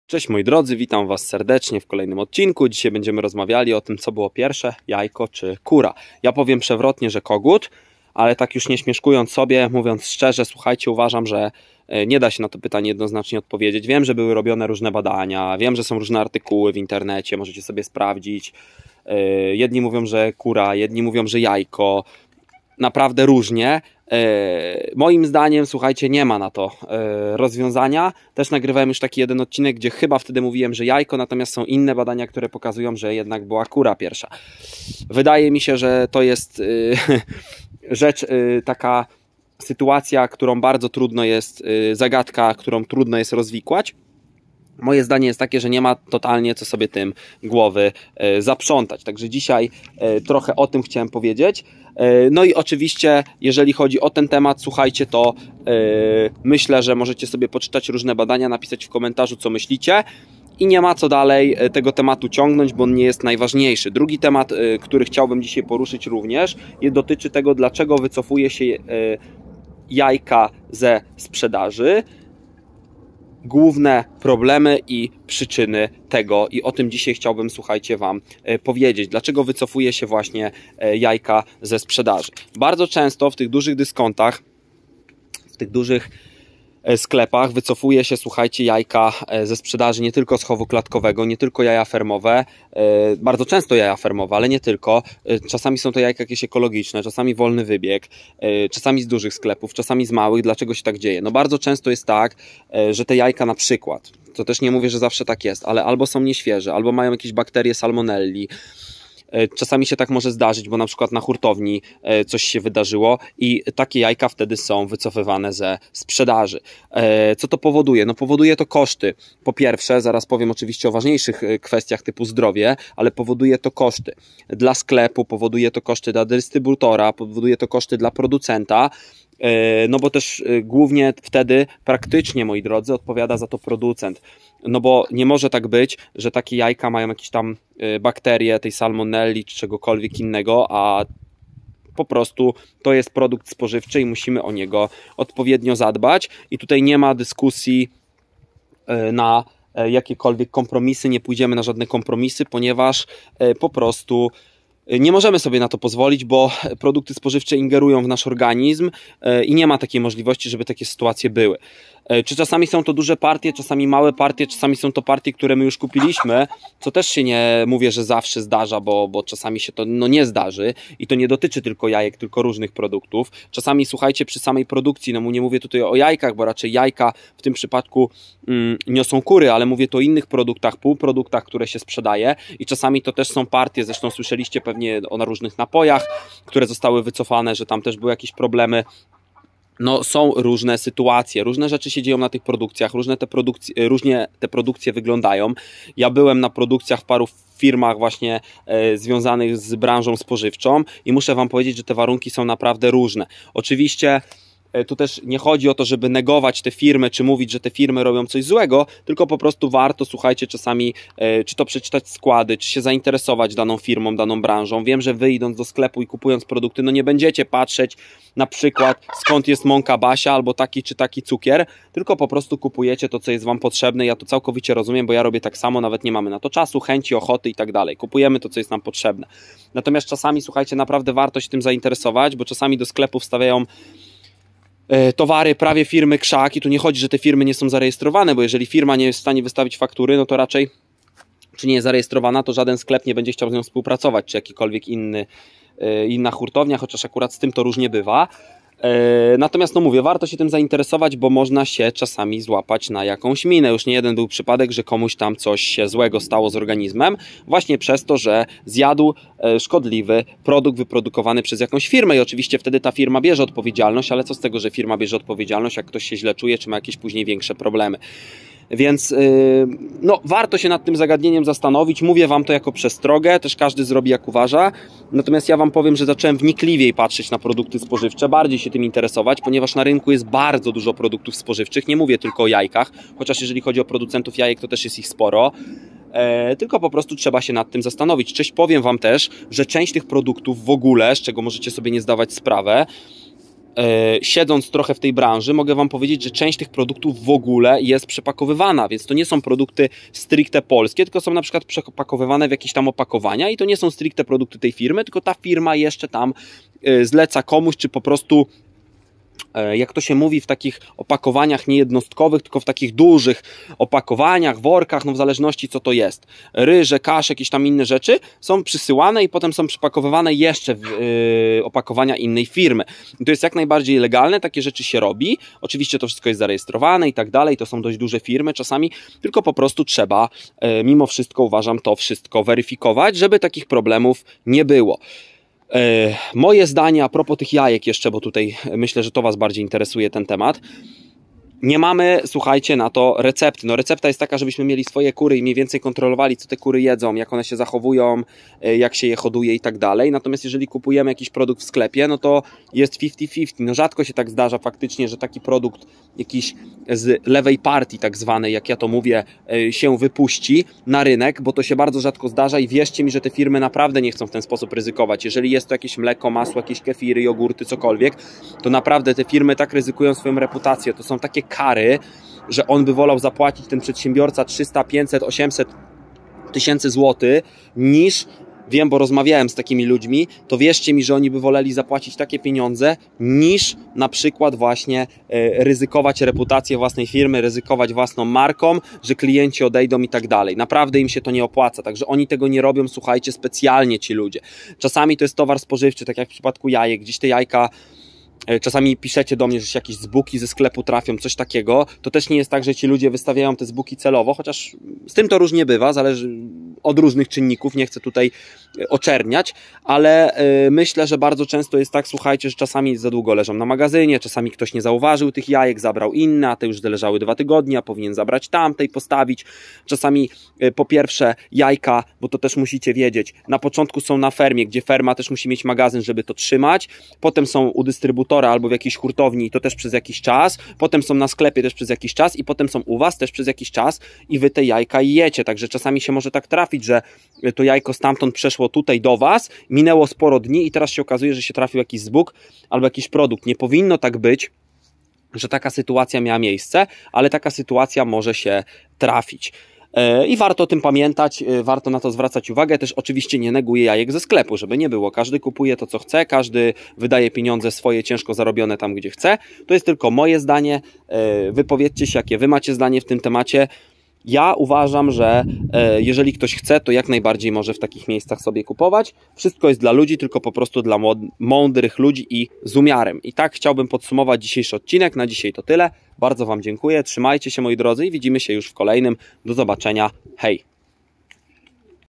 Odcinek 1006 CO BYŁO PIERWSZE JAJKO CZY KURA? DLACZEGO WYCOFUJE SIĘ JAJKA ZE SPRZEDAŻY? PRZYCZYNY
W odcinku porozmawiamy na pare tematów, głównie związanych ze sprzedażą jajek, ale nie tylko.